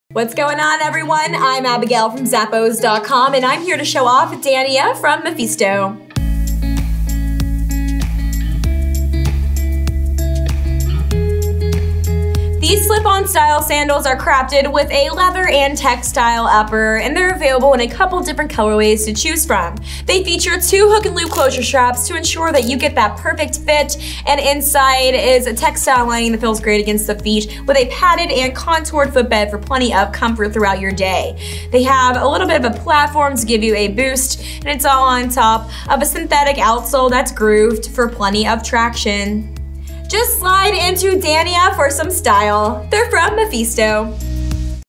Mephisto Dania SKU: 9368799
Step out in style this season when you flaunt the bold and fresh leather Mephisto&reg; Dania platform sandals featuring a breathable leather and textile lining and lightly cushioned, stationed insole.
Easy slip-on design with hook-and-loop closure.
Round toe with platform heel.
Textured synthetic sole.
Imported.

Product measurements were taken using size 38 (US Women's 8), width B - Medium. Please note that measurements may vary by size.